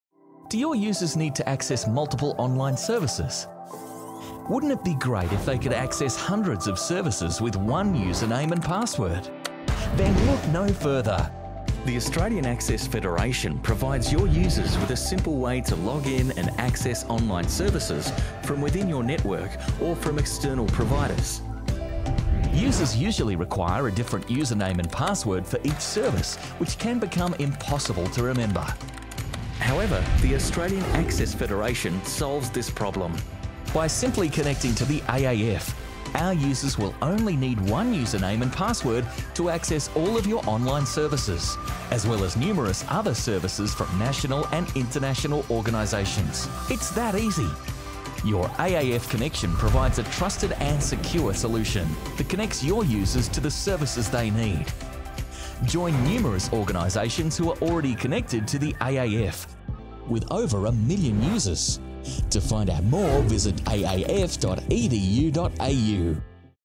What is the AAF?
Find out more about the Australian Access Federation (AAF) and how it could assist your organisation to connect your users to hundreds of services using one username and password.